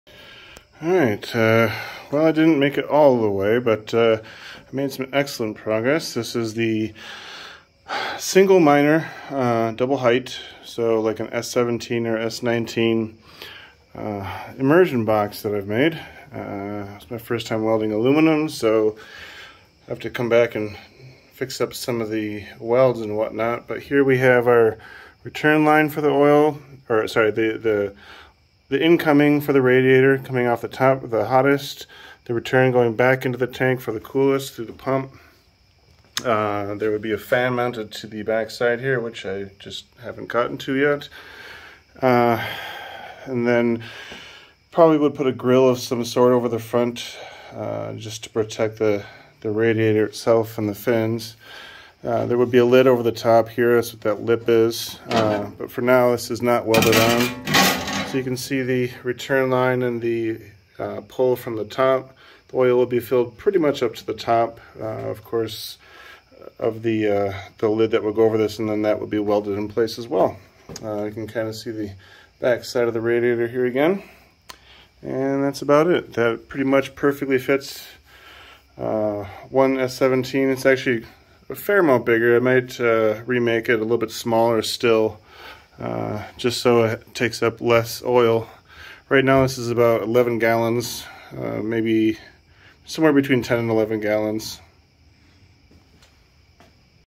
Coin Heated - Hotbox Solo™ single ASIC oil submersion enclosure
This is the first rendition of what will become the Hotbox Solo™, an enclosure designed to house a single ASIC miner of "double" height such as the Antminer S17 or S19.  Total oil capacity will be less than 10 gallons, though this initial build ended up at about 10.5.  Further refinements will be made before a production ready unit is available.  The goal is to be able to have this be able to run indoors with noise no louder than a standard household fan.  There will also be an option to include WiFi, and be able to be placed outdoors, and be weather resistant.